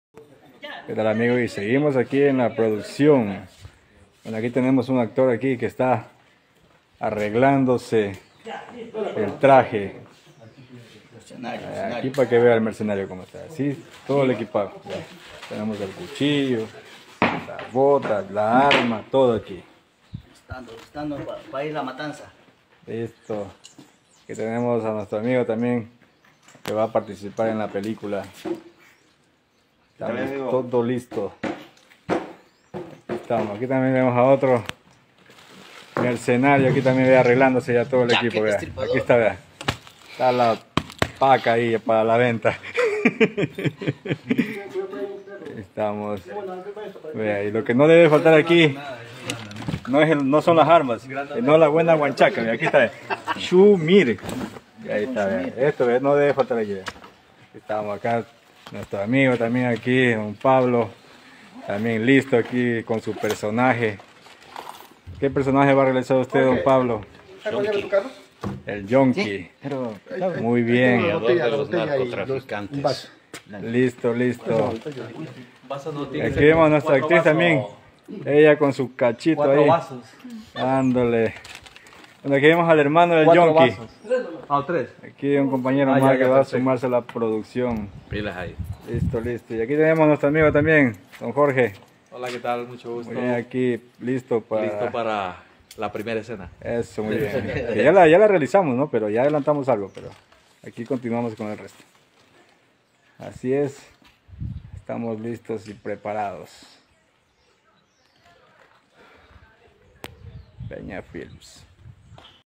Nueva produccion filmografica ,04-12-2022
Seguimos con lo que nos gusta la actucion y muy pronto estaremos estrenando una película realizada en Santo Domingo Gracias a los actores por ser parte de esta película ( PEÑA PRODUCCIONES )